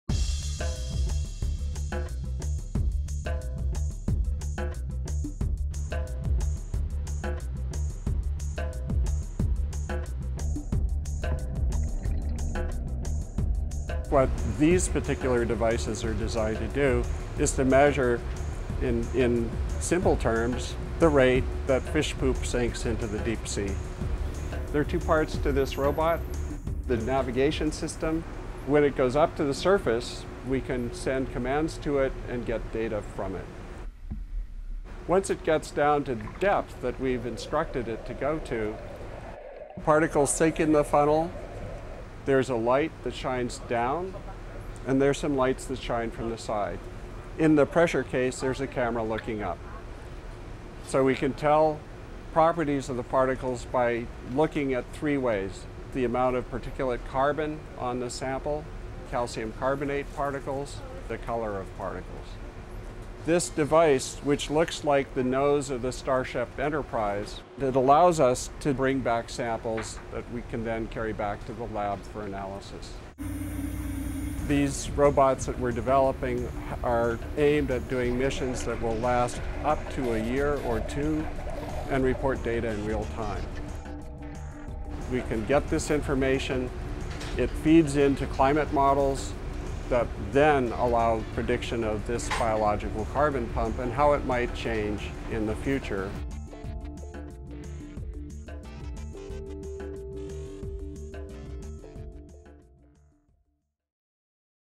Aboard the R/V Oceanus: Carbon Flux Explorers
Jim Bishop, senior scientist at Berkeley Lab and professor at UC Berkeley, is leading a project to deploy robotic floats that provide data on how microorganisms sequester carbon in the ocean. He recently led a research team on a 10-day voyage, funded by the National Science Foundation, to put the Carbon Flux Explorers to the test. (Video edited by Roxanne Makasdjian and Stephen McNally/UC Berkeley. Video shot by Jessica Kendall-Bar. Drone footage provided by Mark Booher.)